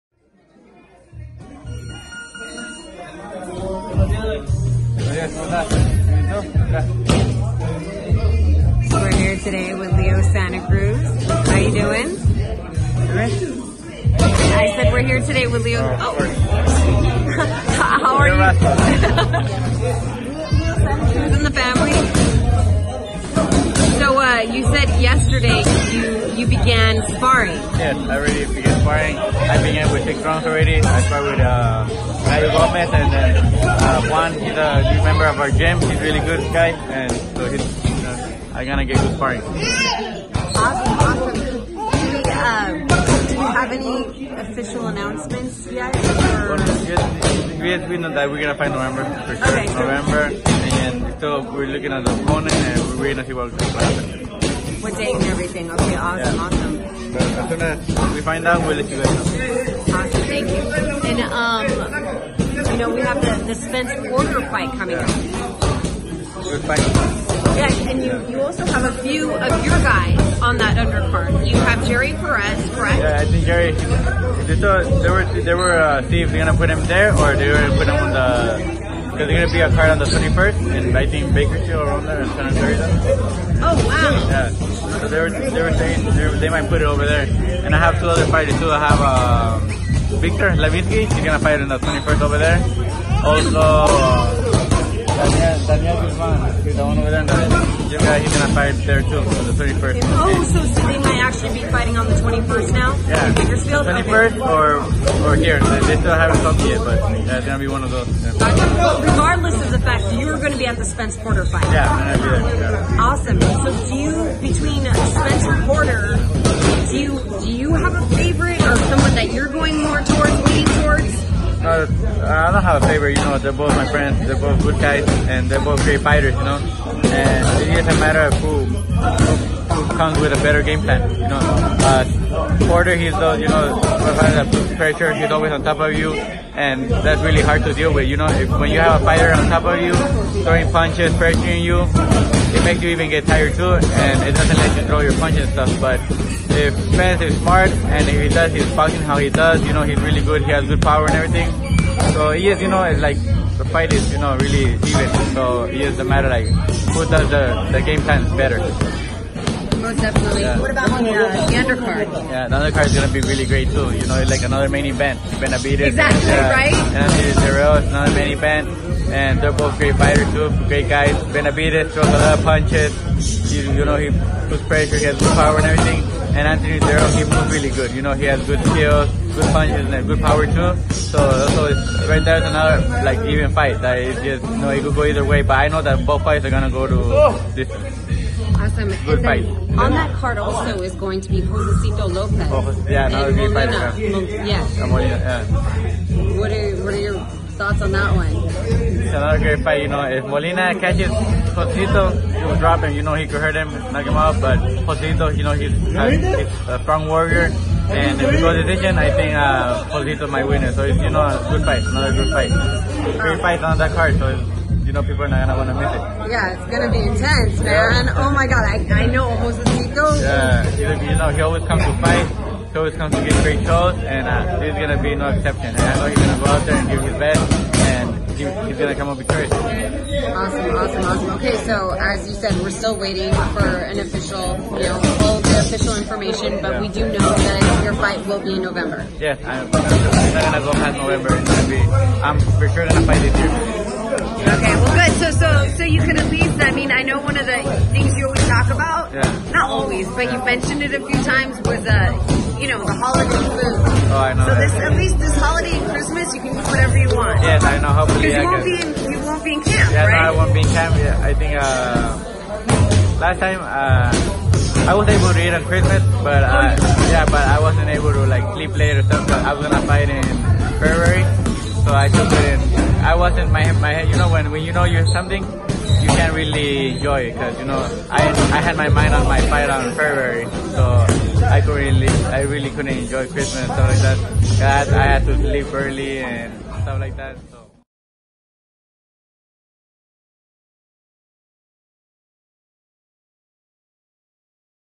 LEO SANTA CRUZ BREAKS DOWN SPENCE VS. PORTER & BENAVIDEZ VS. DIRRELL; EXPECTS GOOD "DISTANCE" FIGHTS
FightHype.com recently caught up with multi-division world champion Leo Santa Cruz, who is in camp staying sharp as he awaits news on his anticipated ring return. You don't want to miss how he looked and what he had to say about several topics. Check it out!